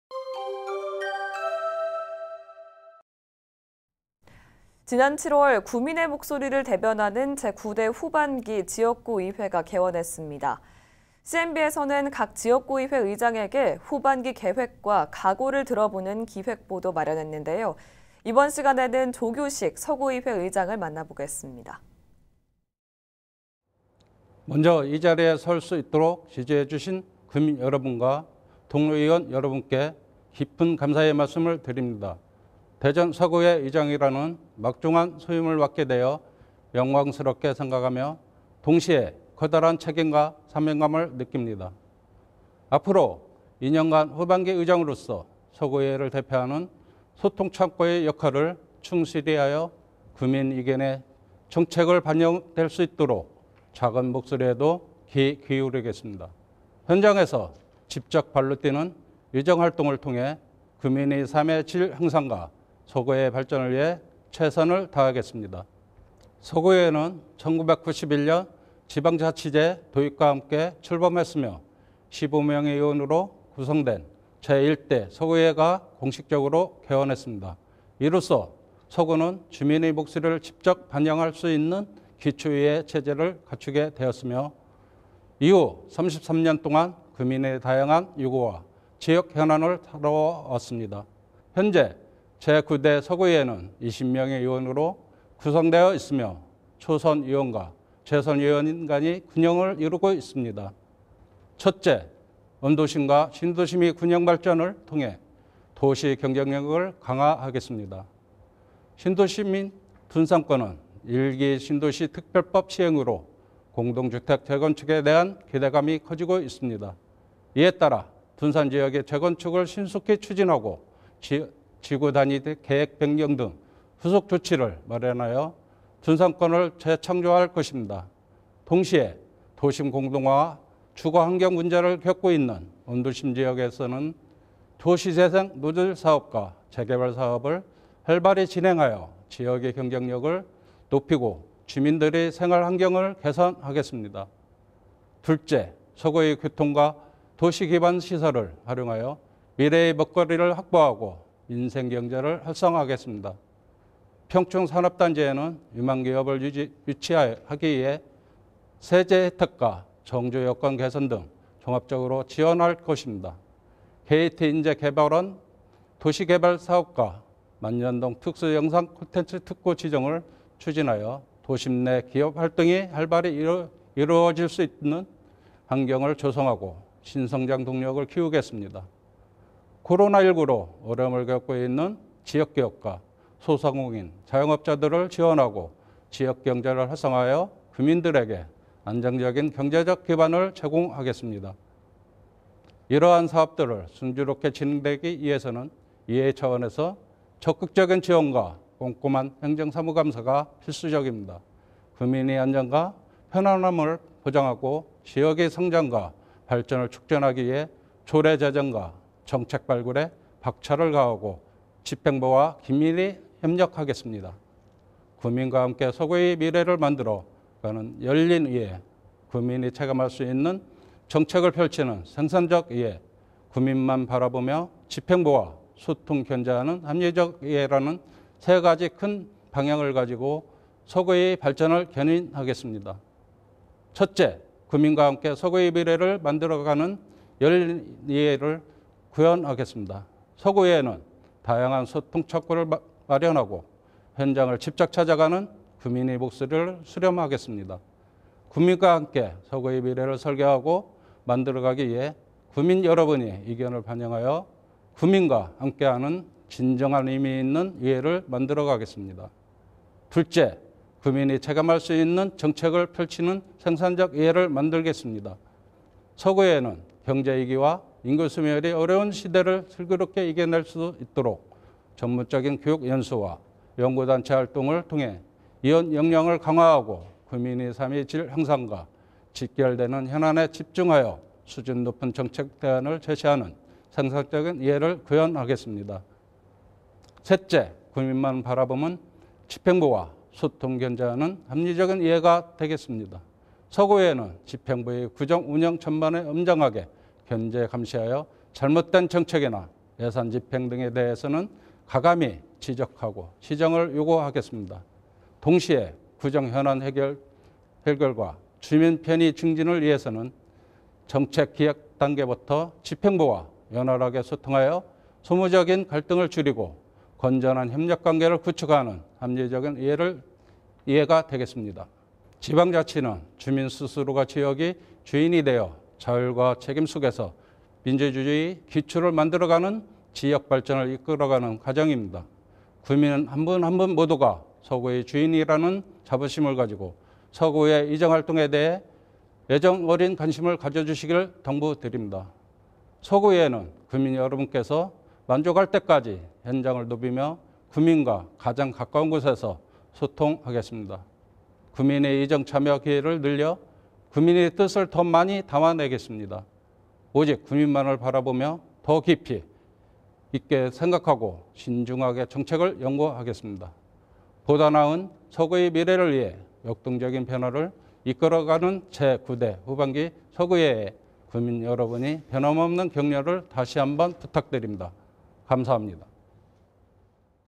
[대전뉴스] [CMB 기획보도] "구민 중심의 의정 실현" - 대전 서구의회
[아나운서]
지난 7월 구민의 대의기관으로서 구민의 목소리를 대변하는 제9대 후반기 지역구의회가 개원했습니다. CMB에서는 각 지역구의회 의장으로부터 후반기 계획과 활동 각오를 들어보는 기획보도 시간을 마련했는데요. 이번에는 조규식 서구의회 의장을 만나봤습니다.

[기사]
▶ 조규식 / 대전광역시 서구의회 의장

먼저 이 자리에 설 수 있도록 지지해 주신 구민 여러분과 동료 의원 여러분께 깊은 감사의 말씀을 드립니다. 대전 서구의회 의장이라는 막중한 소임을 맡게 되어 영광스럽게 생각하며, 동시에 커다란 책임감과 사명감을 느낍니다. 앞으로 2년간 후반기 의장으로서 서구의회를 대표하는 소통 창구의 역할을 충실히 하여 구민의 의견이 정책에 반영될 수 있도록 작은 목소리에도 귀 기울이겠습니다. 현장에서 직접 발로 뛰는 의정 활동을 통해 구민의 삶의 질 향상과 서구의 발전을 위해 최선을 다하겠습니다.

서구의회는 1991년 지방자치제 도입과 함께 출범했으며, 15명의 의원으로 구성된 제1대 서구의회가 공식적으로 개원했습니다. 이로써 서구는 주민의 목소리를 직접 반영할 수 있는 기초의회 체제를 갖추게 되었으며, 이후 33년 동안 구민의 다양한 요구와 지역 현안을 다루어 왔습니다. 현재 제9대 서구의회는 20명의 의원으로 구성되어 있으며, 초선 의원과 재선 의원 간의 균형을 이루고 있습니다.

첫째, 원도심과 신도심의 균형 발전을 통해 도시 경쟁력을 강화하겠습니다. 신도심인 둔산권은 1기 신도시 특별법 시행으로 공동주택 재건축에 대한 기대감이 커지고 있습니다. 이에 따라, 둔산 지역의 재건축을 신속히 추진하고 지구단위계획 변경 등 후속 조치를 마련하여 둔산권을 재창조할 것입니다. 동시에, 도심 공동화와 주거 환경 문제를 겪고 있는 원도심 지역에서는 도시재생 뉴딜사업과 재개발 사업을 활발히 진행하여 지역의 경쟁력을 높이고 주민들의 생활환경을 개선하겠습니다. 둘째, 서구의 교통과 도시기반 시설을 활용하여 미래 먹거리를 확보하고 민생경제를 활성화하겠습니다. 평촌 산업단지에는 유망 기업을 유치하기 위해 세제 혜택과 정주 여건 개선 등을 종합적으로 지원할 것입니다. KT인재개발원 도시개발사업과 만년동 특수영상콘텐츠특구 지정을 추진하여 도심 내 기업 활동이 활발히 이루어질 수 있는 환경을 조성하고 신성장 동력을 키우겠습니다. 코로나19로 어려움을 겪고 있는 지역 기업과 소상공인·자영업자들을 지원하고 지역 경제를 활성화하여 구민들에게 안정적인 경제적 기반을 제공하겠습니다. 이러한 사업들이 순조롭게 진행되기 위해서는 의회 차원의 적극적인 지원과 꼼꼼한 행정사무감사가 필수적입니다. 구민의 안전과 편안함을 보장하고 지역의 성장과 발전을 촉진하기 위해 조례 제정과 정책 발굴에 박차를 가하고, 집행부와 긴밀히 협력하겠습니다.

구민과 함께 서구의 미래를 만들어 가는 ‘열린 의회’, 구민이 체감할 수 있는 정책을 펼치는 ‘생산적인 의회’, 구민만 바라보며 집행부와 소통·견제하는 ‘합리적인 의회’라는 세 가지 큰 방향을 가지고 서구의 발전을 견인하겠습니다. 첫째, 구민과 함께 서구의 미래를 만들어 가는 ‘열린 의회’를 구현하겠습니다. 서구의회는 다양한 소통 창구를 마련하고 현장을 직접 찾아가 구민의 목소리를 수렴하겠습니다. 구민과 함께 서구의 미래를 설계하고 만들어가기 위해 구민 여러분의 의견을 반영하여 구민과 함께하는 진정한 의미의 의회를 만들어가겠습니다. 둘째, 구민이 체감할 수 있는 정책을 펼치는 ‘생산적인 의회’를 만들겠습니다. 서구의회는 경제 위기와 인구 소멸의 어려운 시대를 슬기롭게 이겨낼 수 있도록 전문적인 교육 연수와 연구 단체 활동을 통해 의원 역량을 강화하고 구민의 삶의 질 향상과 직결되는 현안에 집중하여 수준 높은 정책 대안을 제시하는 생산적인 의회를 구현하겠습니다. 셋째, 구민만 바라보며 집행부와 소통·견제하는 ‘합리적인 의회’가 되겠습니다. 서구의회는 집행부의 구정 운영 전반을 엄정하게 견제·감시하여 잘못된 정책이나 예산 집행 등에 대해서는 과감히 지적하고 시정을 요구하겠습니다. 동시에 구정 현안 해결과 주민 편의 증진을 위해서는 정책기획 단계부터 집행부와 원활하게 소통하여 소모적인 갈등을 줄이고 건전한 협력관계를 구축하는 합리적인 의회가 되겠습니다.

지방자치는 주민 스스로가 지역의 주인이 되어 자율과 책임 속에서 민주주의의 기초를 만들어가고 지역 발전을 이끌어가는 과정입니다. 구민 한 분 한 분 모두가 서구의 주인이라는 자부심을 가지고 서구의회 의정활동에 대해 애정 어린 관심을 가져주시기를 당부드립니다. 서구의회는 구민 여러분께서 만족하실 때까지 현장을 누비며 구민과 가장 가까운 곳에서 소통하겠습니다. 구민의 의정 참여 기회를 늘려 구민의 뜻을 더 많이 담아내겠습니다. 오직 구민만을 바라보며 더 깊이 있게 생각하고 신중하게 정책을 연구하겠습니다. 보다 나은 서구의 미래를 위해 역동적인 변화를 이끌어가는 제9대 후반기 서구의회에 구민 여러분의 변함없는 격려를 다시 한번 부탁드립니다. 감사합니다.